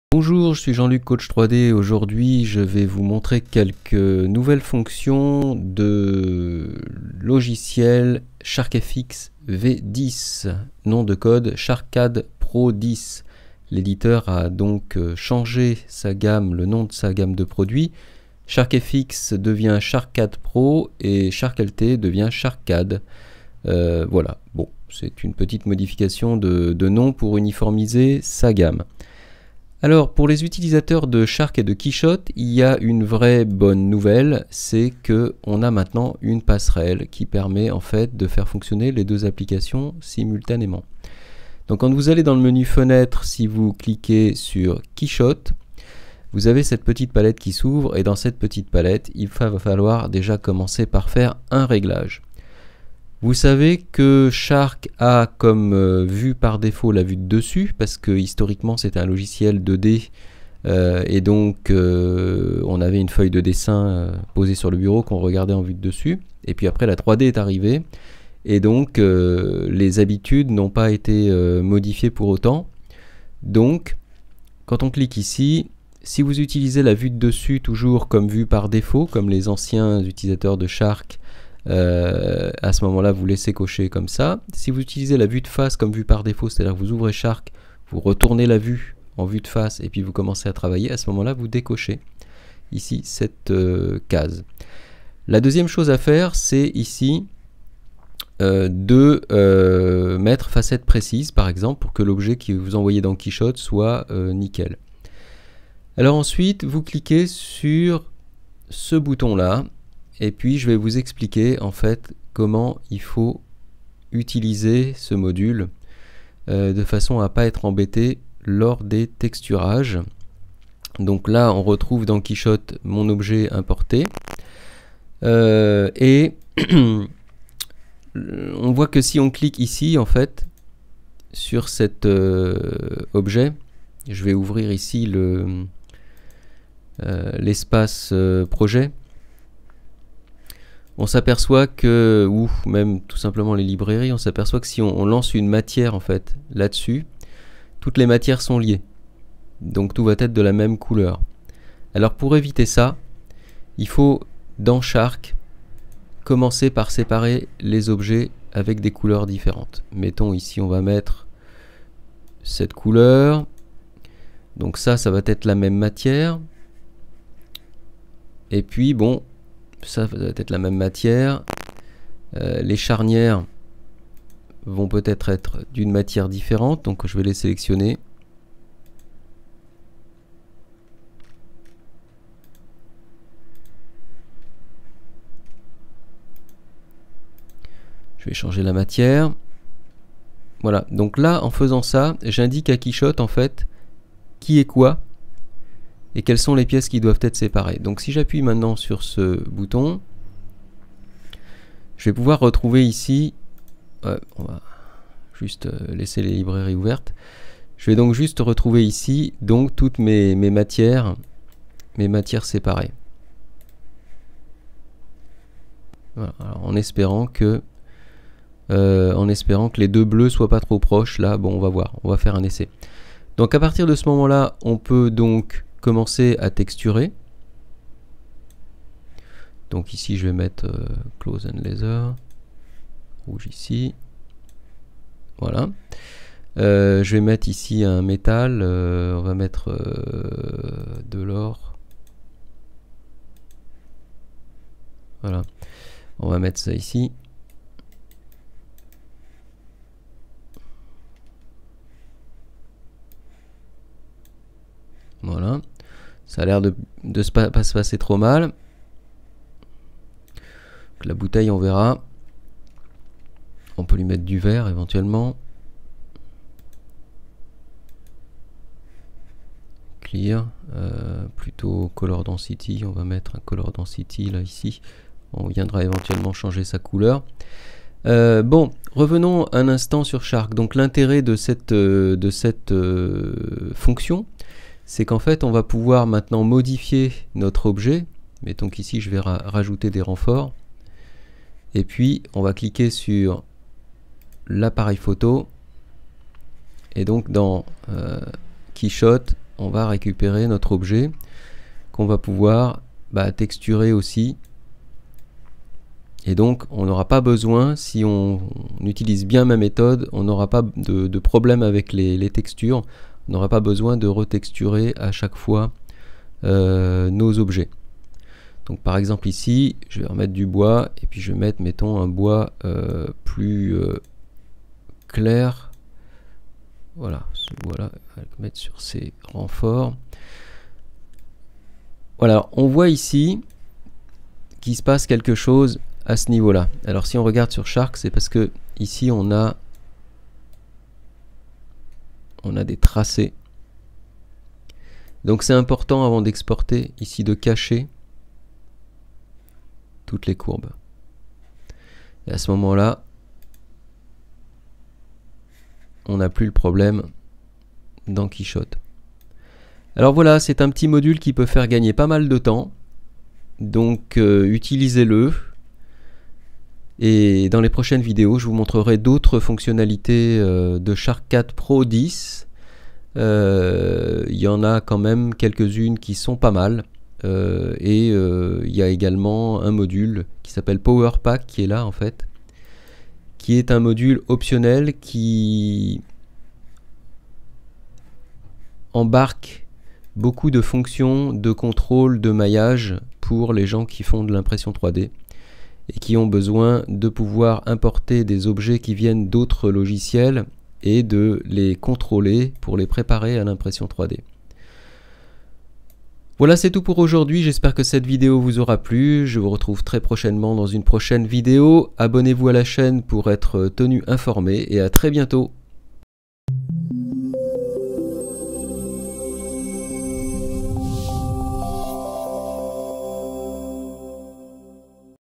SharkCADPro 10 Sychnronisation Keyshot
Dans cette video je vous montre comment synchroniser Shark FX V10 ( SharkCADPro 10 avec Keyshot )